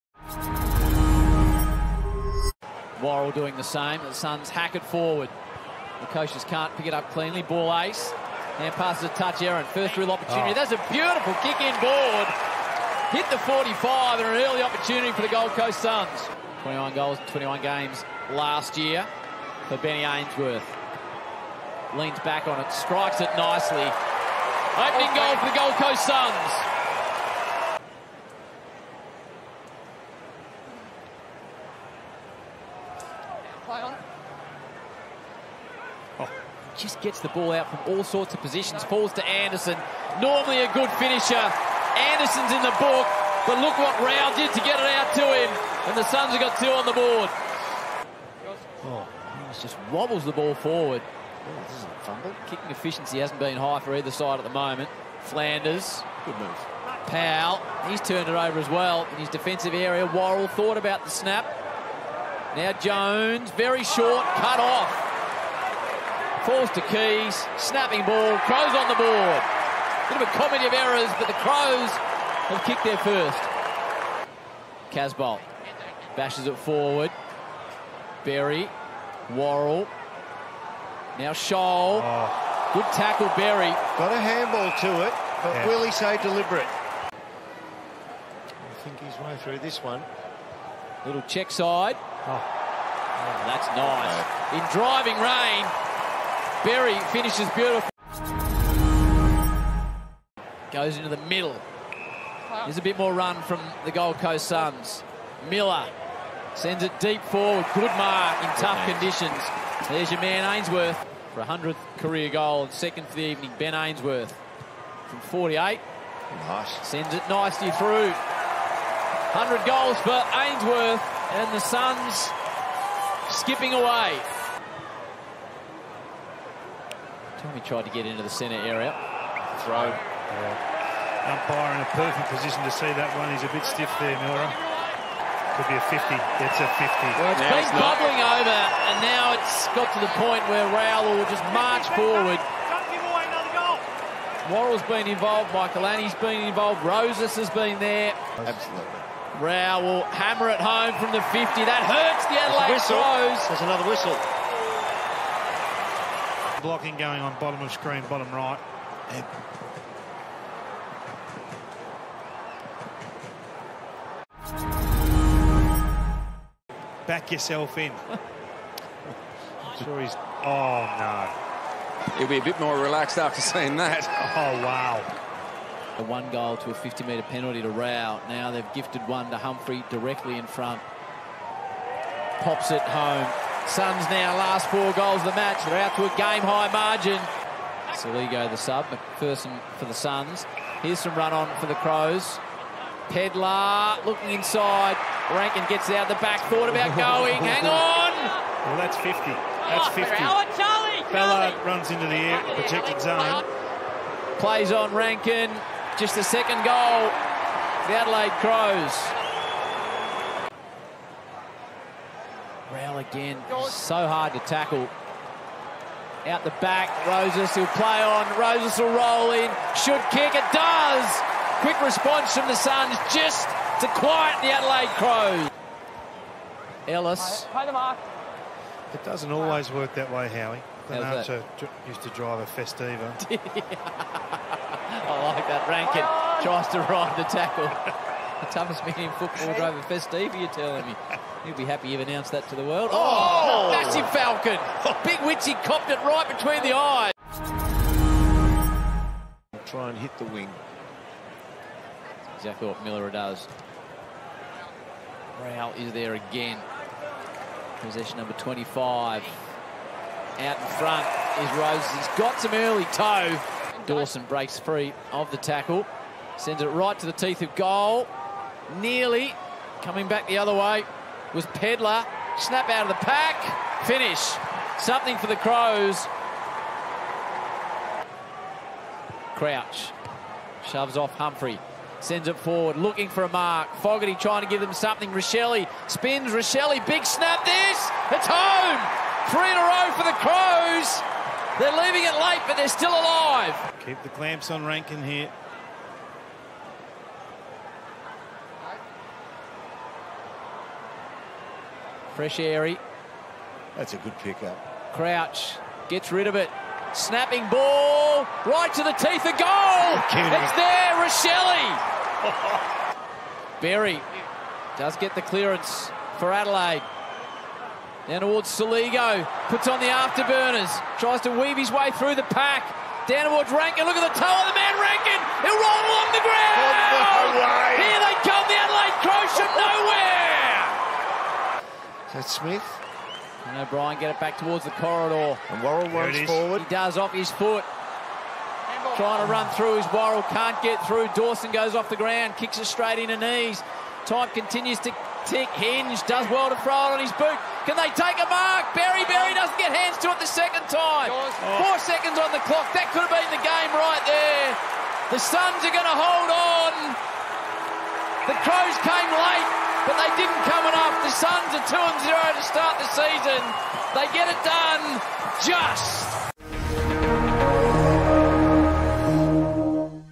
Highlights R1: v Gold Coast Suns
Rewatch the highlights from our round one clash against the Gold Coast Suns.